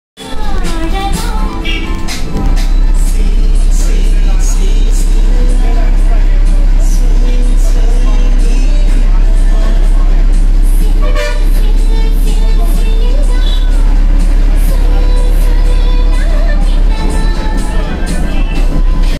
Tamil Nadu State Transport Corporation Bus Kanyakumari to Nagercoil | Last seat view less passengers
New fleet of red buses